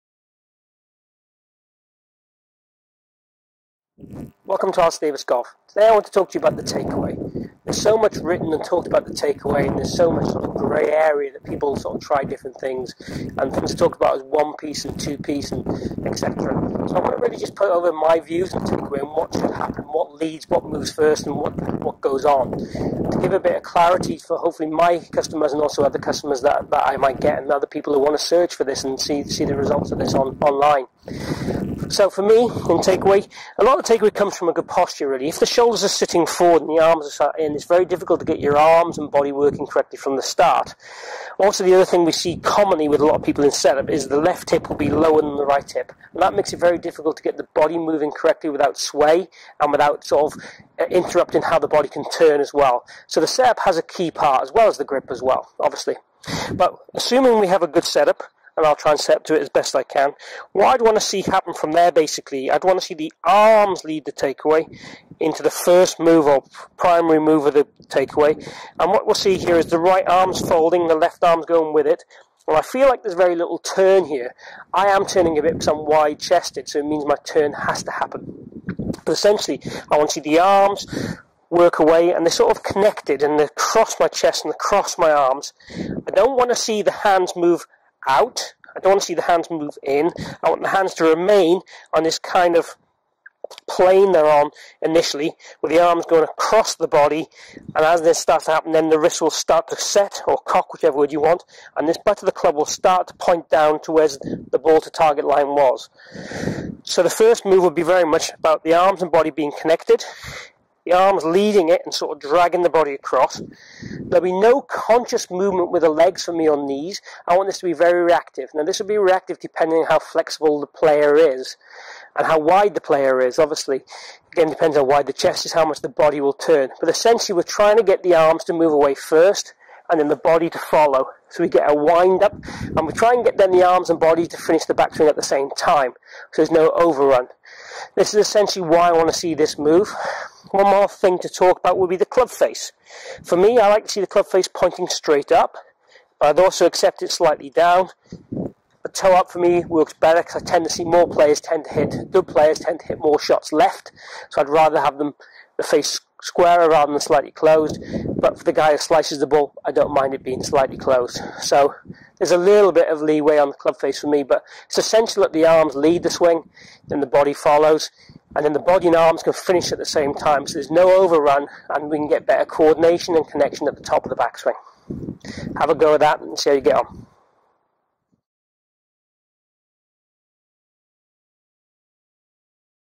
The Takeaway| How To Start The Swing Correctly
Golf Tip - The Takeaway - How to start the swing correctly. Alistair Davies golf explains what starts the golf swing - the takeaway. Simple explanation of what moves first in the golf swing and why.